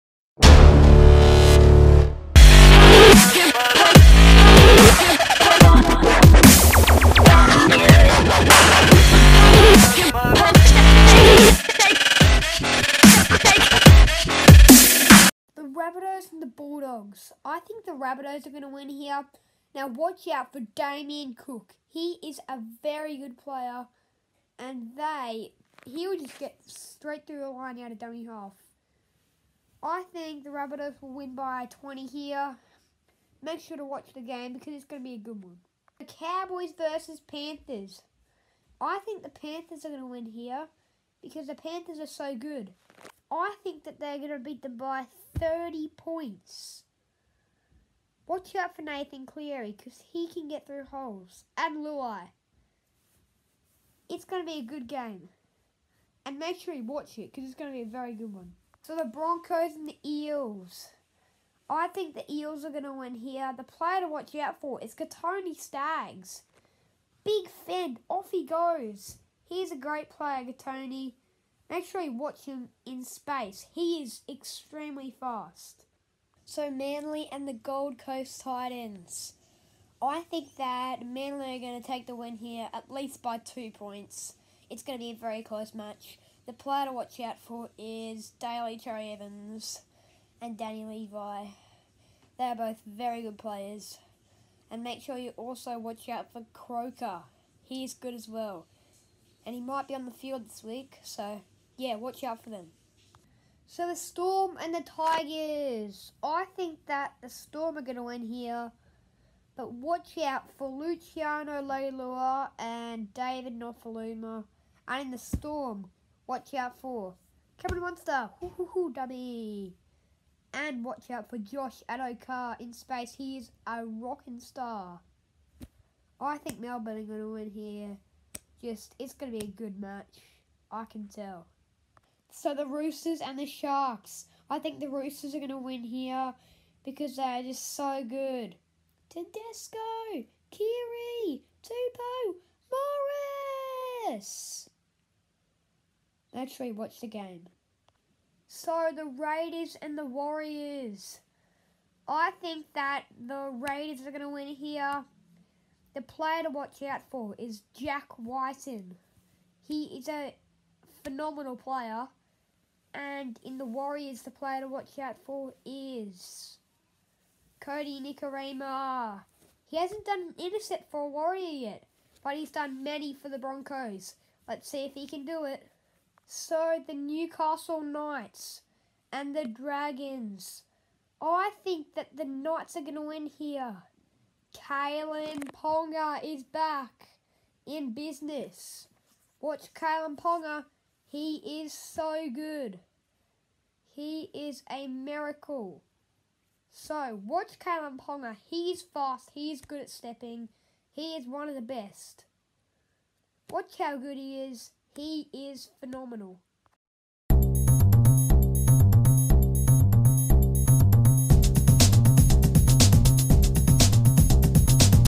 NRL 2020 Round 19 Tips
This week we talk about some of the exciting players of round 19, tips and predictions.